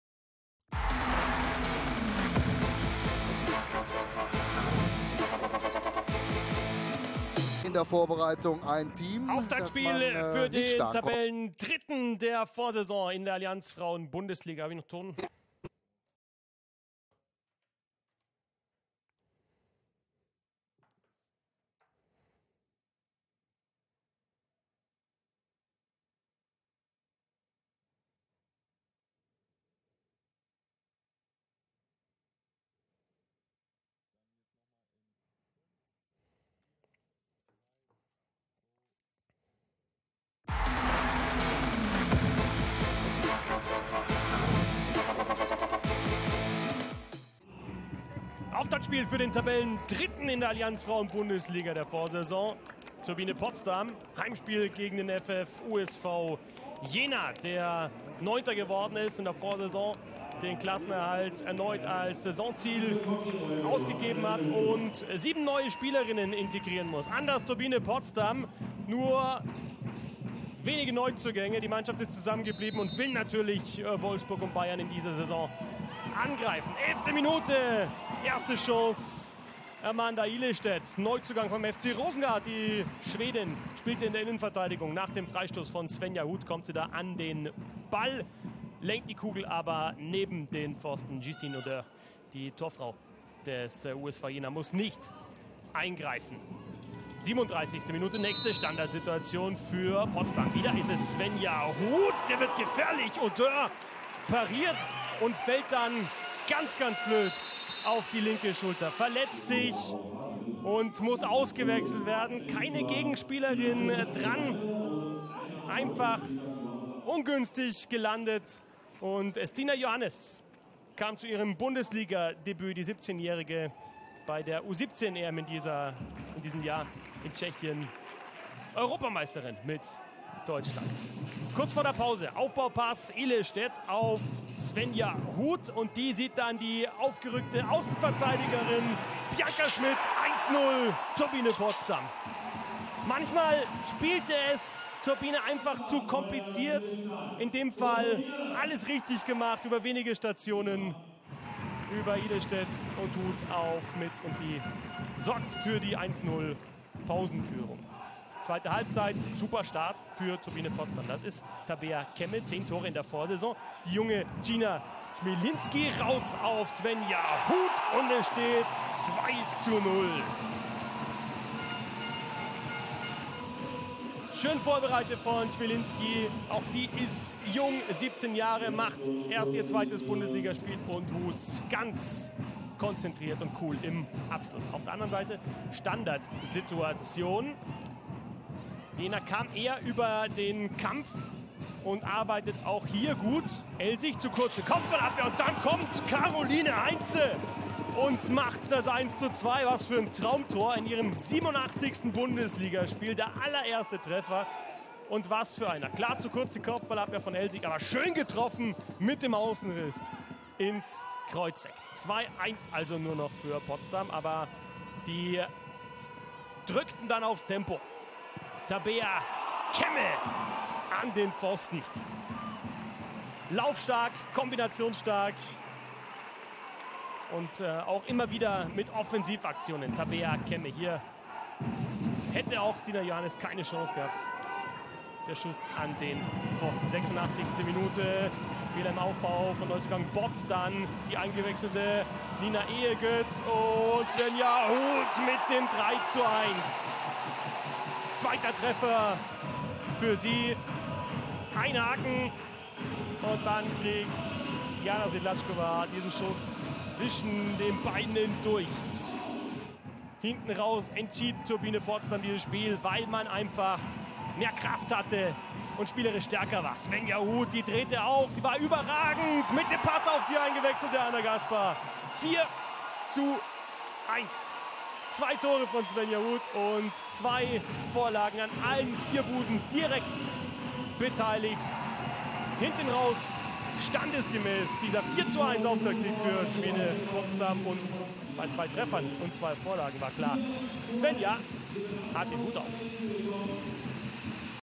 1. FFC Turbine Potsdam - FF USV Jena | 01. Spieltag, 17/18 | Telekom Sport
Turbine Potsdam gelingt mit einem 4:1 Sieg gegen Jena der erfolgreiche Saisonstart.

Zu Beginn wirkten beide Teams sehr unsicher und auch ein wenig fahrig. Die Zuspiele waren ungenau und auch die Abstimmungen zwischen den Spielerinnen waren noch nicht ganz klar.
Trotz der anfänglichen Ungenauigkeiten hagelte es trotzdem Chancen für Turbine, doch die Potsdamerinnen mussten bis zur 43. Minute auf die Erlösung warten. Bianca Schmidt traf für die Potsdamerinnen zum 1:0. Die weiteren Treffer erzielten Svenja Huth und Anna Gasper. Für Jena traf Karoline Heinze zum zwischenzeitlichem 2:1.

Aufstellungen:

Turbine Potsdam:
Schmitz - Schmidt, Ilestedt, Elsig, Kellond-Knight - Cramer, Kemme, Wälti - Rauch, Huth, Chmielinski

FF USV Jena:
Odeurs - Woeller, Zigic, Sedlackova, Heinze - Utes, Graf, Rudelic, King - Rieke, Pietrangelo